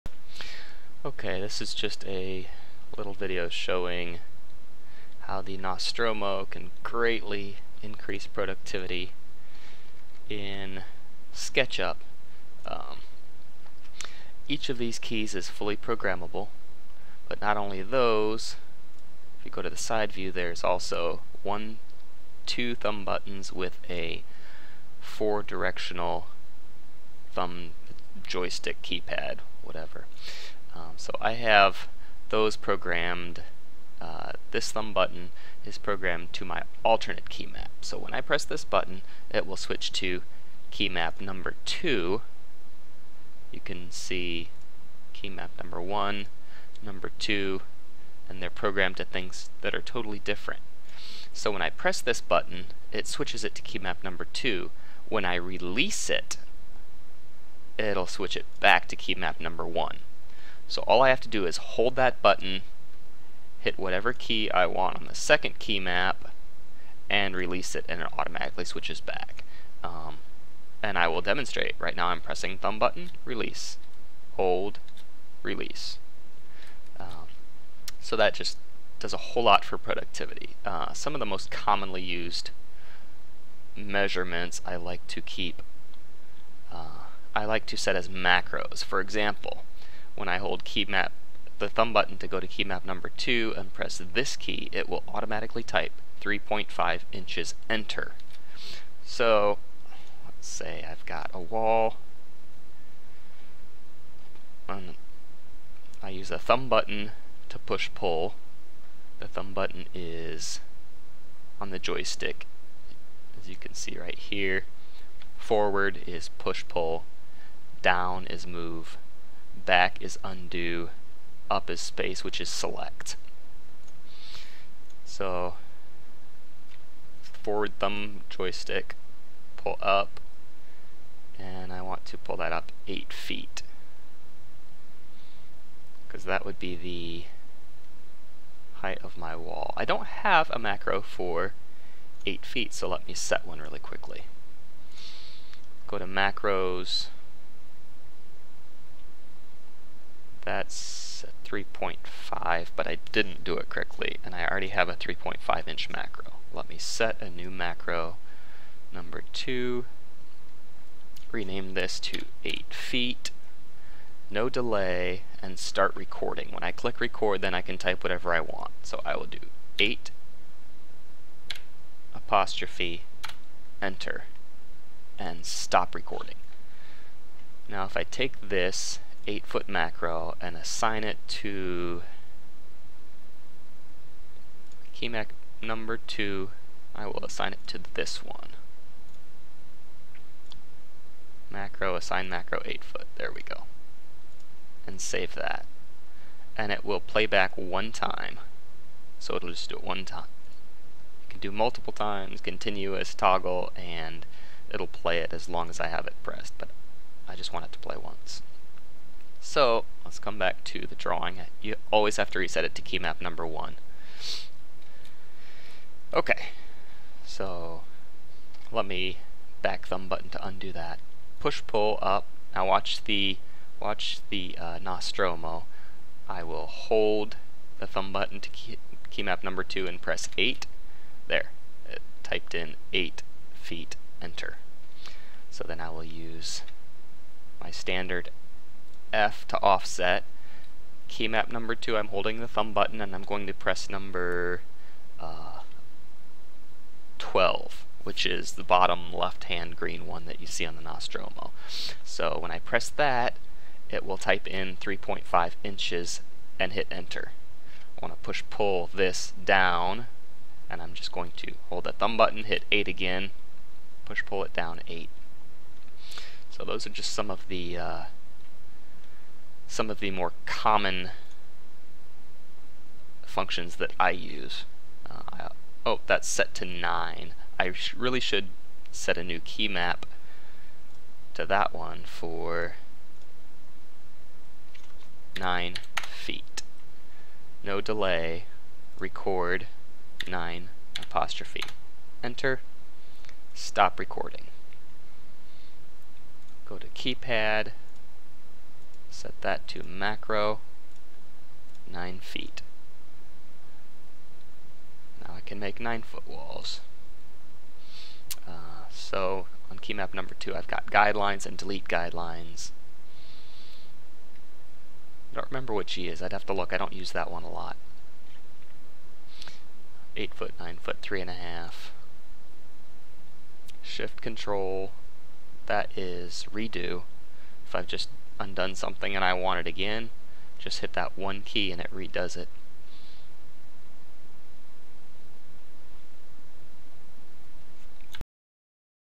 SketchUp Productivity with Nostromo
Using Nostromo to simplify everyday functions in SketchUp.  Adding macros is quick, simple, and keeps you from having to move your right hand off the mouse to enter commonly used measurements.